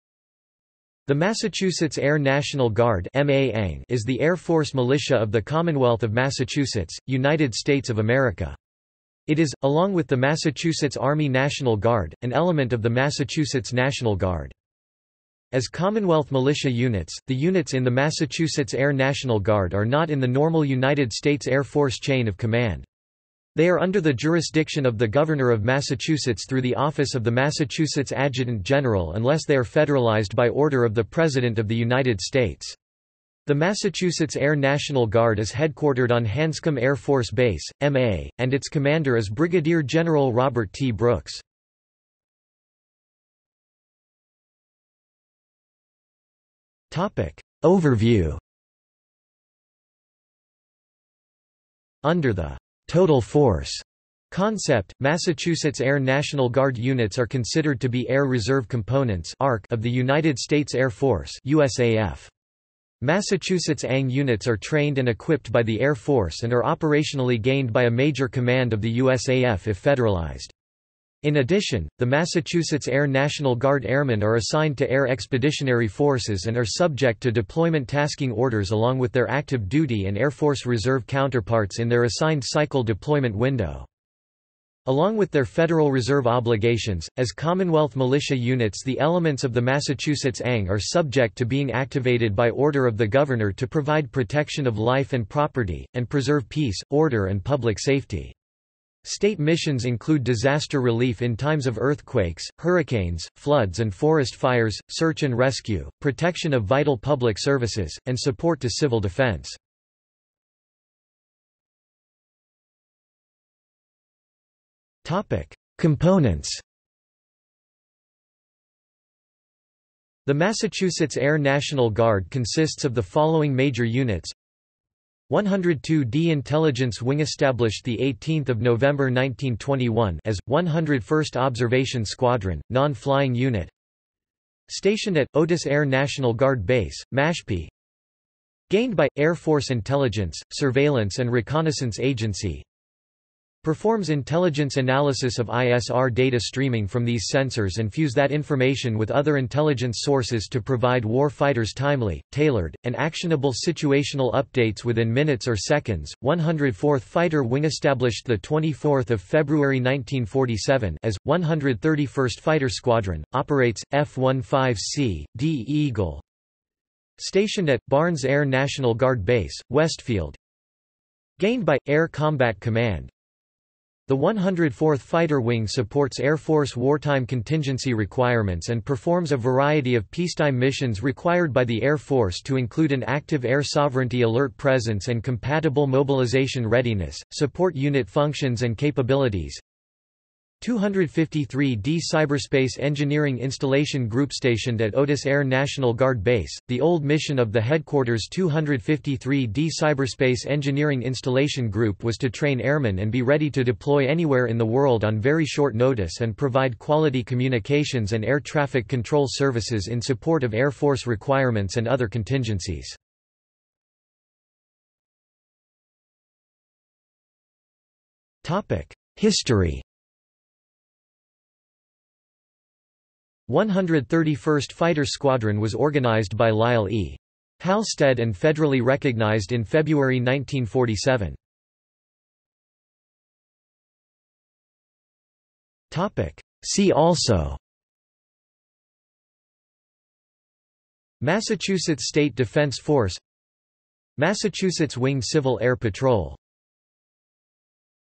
Massachusetts Air National Guard | Wikipedia audio article
This is an audio version of the Wikipedia Article:
Massachusetts Air National Guard

00:00:43 1 Overview
00:01:51 2 Components
00:03:34 3 History
00:03:52 4 See also

- Socrates

SUMMARY
The Massachusetts Air National Guard (MA ANG) is the air force militia of the Commonwealth of Massachusetts, United States of America. It is, along with the Massachusetts Army National Guard, an element of the Massachusetts National Guard.
As commonwealth militia units, the units in the Massachusetts Air National Guard are not in the normal United States Air Force chain of command.  They are under the jurisdiction of the Governor of Massachusetts through the office of the Massachusetts Adjutant General unless they are federalized by order of the President of the United States.  The Massachusetts Air National Guard is headquartered on Hanscom Air Force Base, MA, and its commander is Brigadier General Robert T. Brooks.